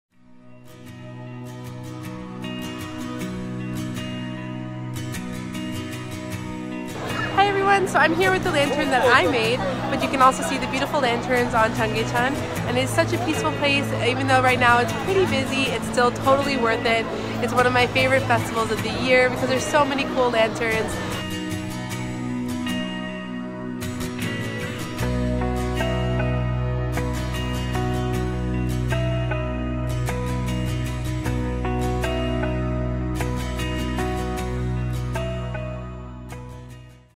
Seoul Lantern Festival 2018
Had a blast at the @seoullanternfestival with my Global Seoul Mates. Don’t miss all the fun. 🏮🏮